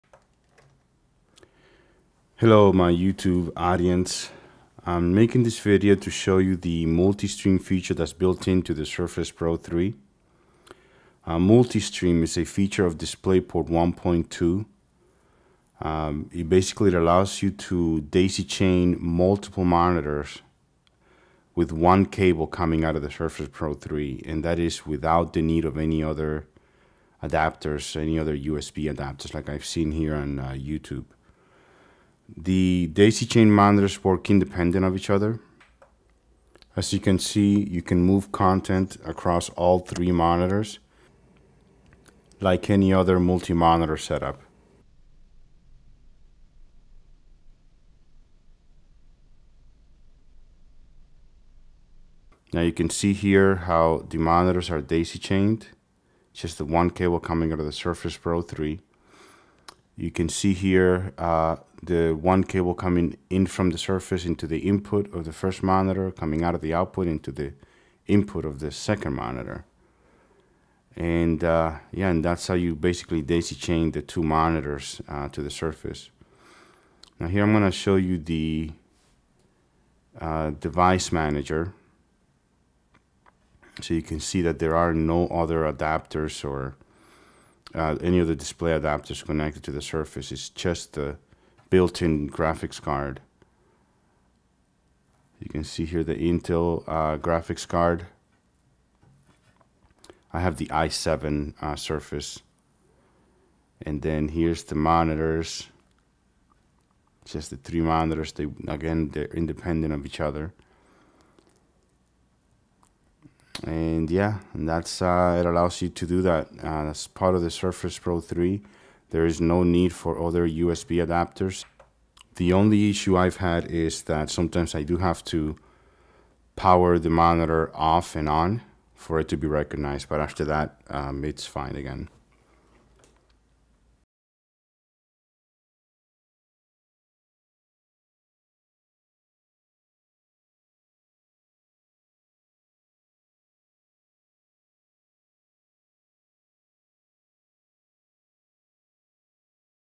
Surface Pro 3 - Multiple Monitors no adapters needed - Multi-Stream
This video is to show the DisplayPort Multi-Stream feature on the Surface Pro 3 - No need for extra USB adapters